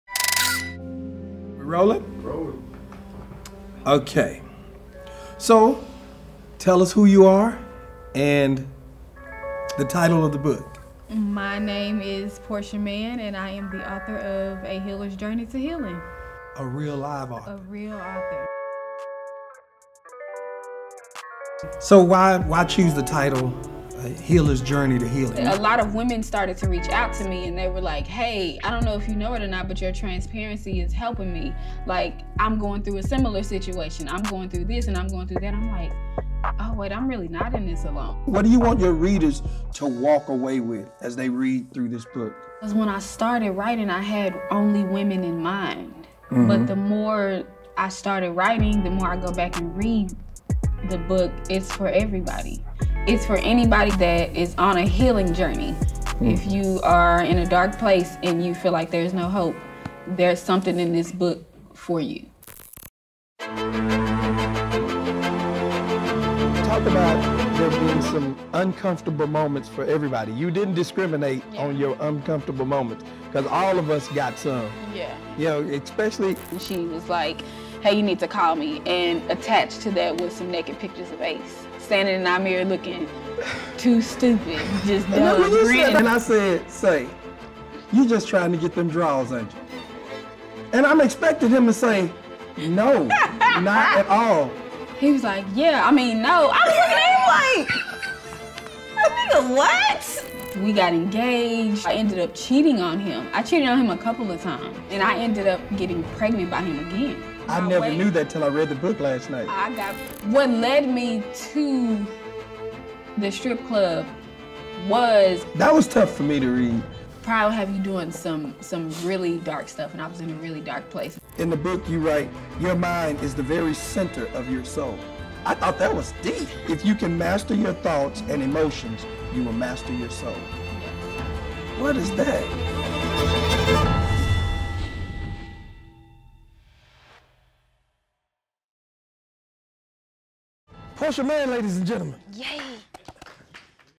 A Healer's Journey to Healing: Porcia Mann
New book, A Healer's Journey to Healing, by Porcia Mann, now available!

“This is a story for anyone who is on a journey to healing or who finds themselves in the middle of breaking generational curses,” says Porcia Mann, social media influencer about her new book, A Healer’s Journey to Healing, now available.

At first, the new author with the famous surname wondered if her story would be overlooked because of her famous parents, NAACP Award-winning actor David Mann and Grammy Award-winning mother, Tamela Mann.

“People think that since my parents have been in the entertainment industry for a very long time, that I didn’t have any troubles,” says Porcia. “But not many people know I have struggled, and that most times I have had to experience things on my own to build character. I was even flat-out rebellious at other times.” Watch Porcia and David get personal about growing up ‘Mann’ in this clip.